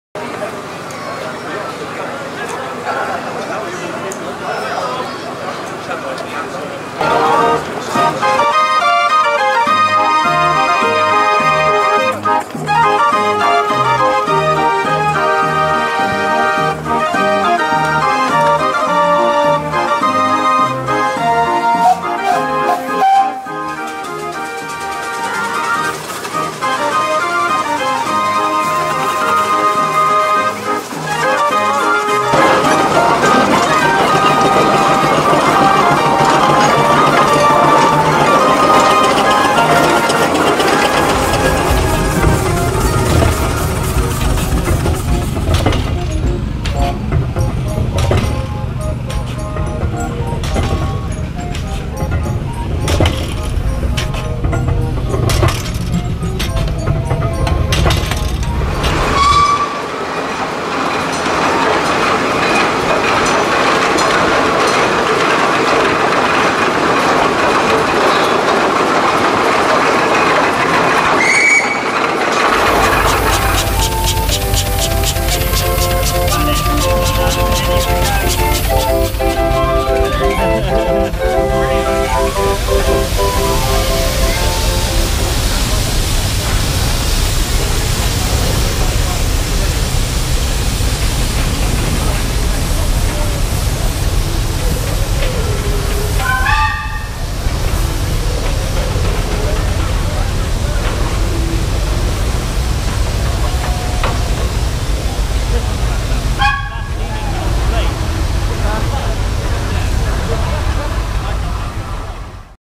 Camborne Trevithick Day 2015 short edit
Camborne Trevithick Day 2015 Longer edit in progress! Videoed at 4k resolution and this will take a few days to finish. Watch this space!! For enthusiasts edit